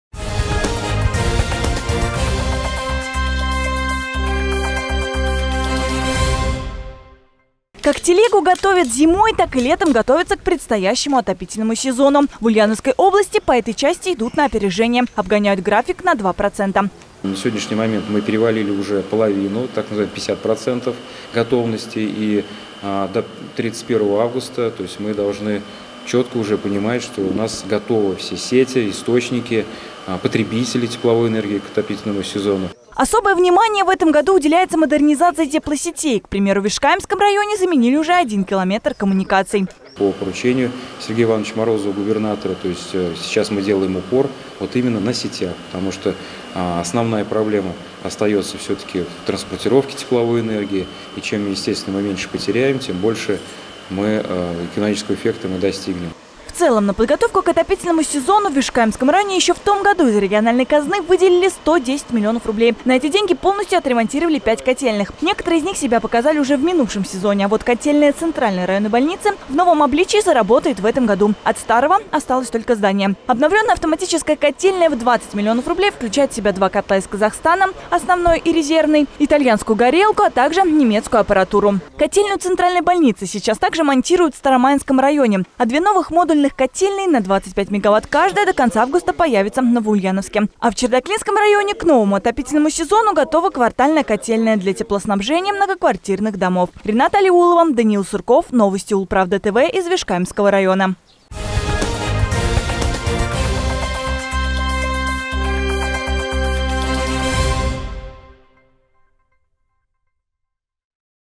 С новым оборудованием и без порывов. В Ульяновской области готовятся к новому отопительному сезону
Как телегу готовят зимой, так  и летом готовятся к предстоящему отопительному сезону. В Ульяновской области по этой части идут на опережение. Обгоняют график на 2 %.